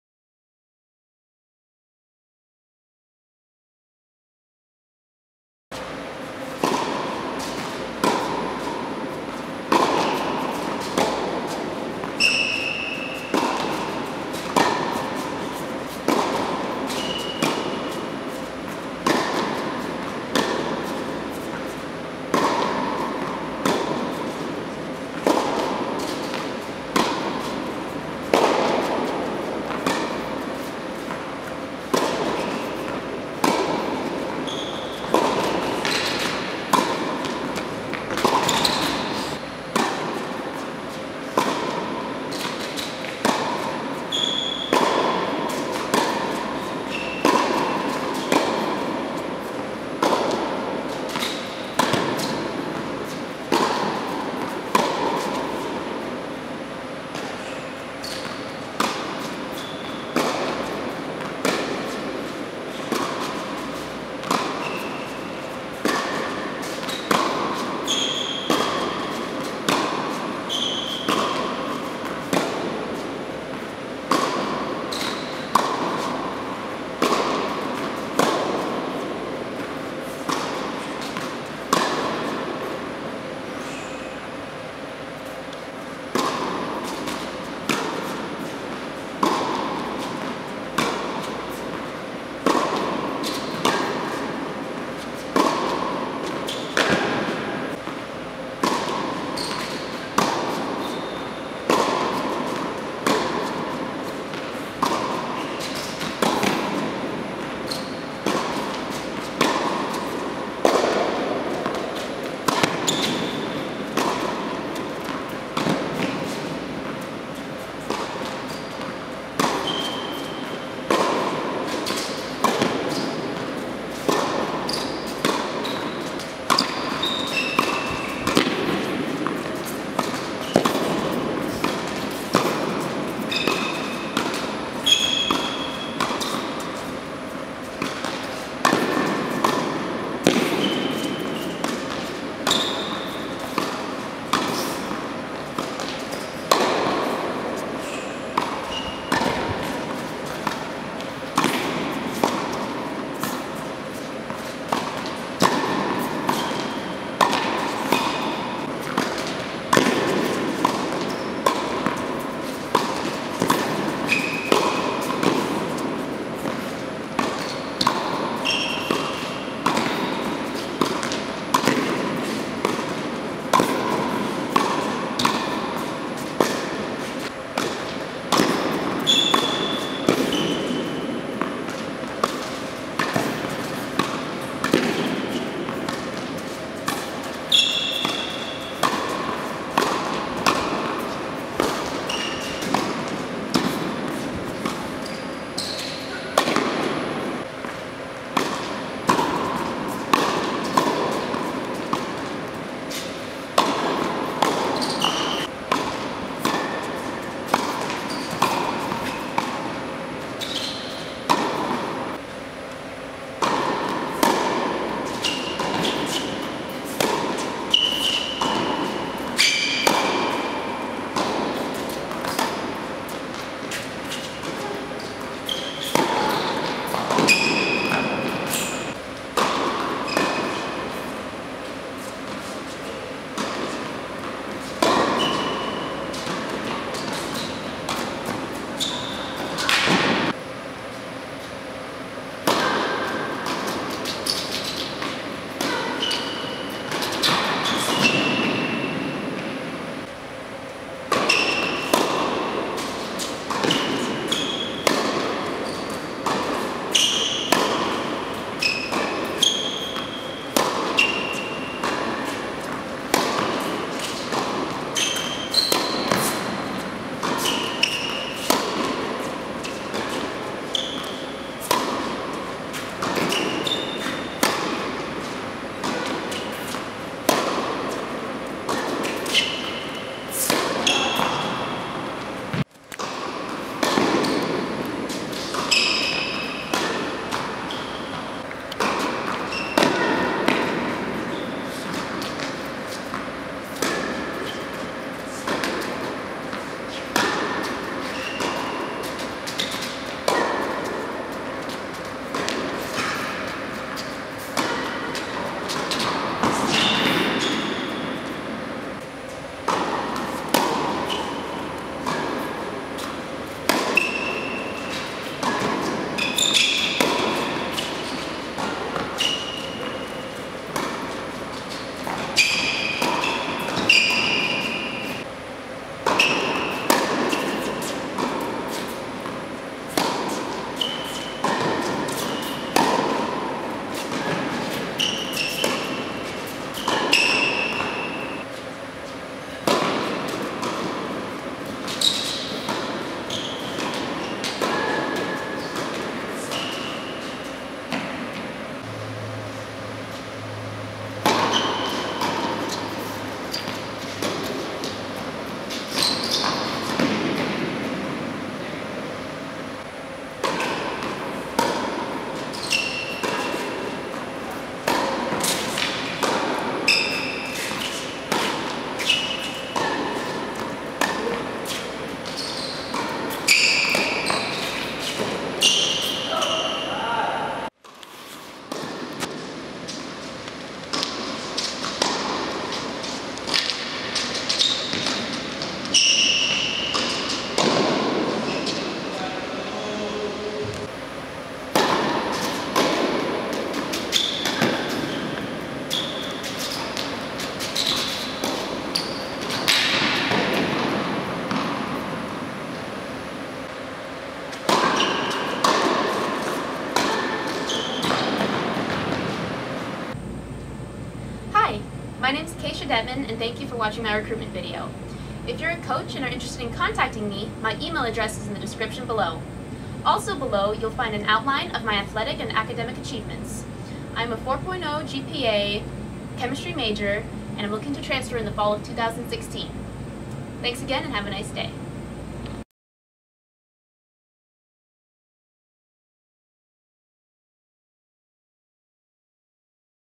College Tennis Recruiting Video - Acacia Dettman 2016
Hello! Below are some details about my college record in both athletics and academics

I recently finished my second year of competition at Riverside City College in Southern California, and I am looking for a school to which I can transfer, continue my education, and continue my college tennis career.

At RCC, I played at the #1 singles position and #1 doubles position the majority of both my freshman and sophomore years, and I was an All-Conference player in both singles and doubles both my freshman and sophomore years.

In 2014, I was ranked #5 in singles and #6 in doubles in Southern California Junior colleges, and finished the year at #14 in singles and #10 in doubles in all of California's Junior colleges.

In 2015, I was ranked #9 in singles and #8 in doubles in Southern California Junior colleges, and I finished #12 in singles and #13 in doubles in all of California's Junior colleges. In my second year on the RCC team, I was appointed team captain and helped my team become Conference Champions, posting a 12-3 record at #1 singles, a 4-0 record at #2 singles, a 9-5 record at #1 doubles, and a 3-0 record at #2 doubles.

I had the honor of receiving the Arthur Ashe Leadership and Sportsmanship award for my region at California’s Ojai State Tennis Tournament. And just recently, I was awarded the “Academic All-State Award” by the California Community College Athletic Association.

Academically, I am majoring in chemistry with the hopes of becoming a forensic scientist. I have completed 61 units, have an overall GPA of 4.0, and I’ve made the Dean’s Honor List every semester I’ve been here. I decided to stay at RCC for an extra year to complete my AS degree, which I will receive by the end of the spring semester of 2016.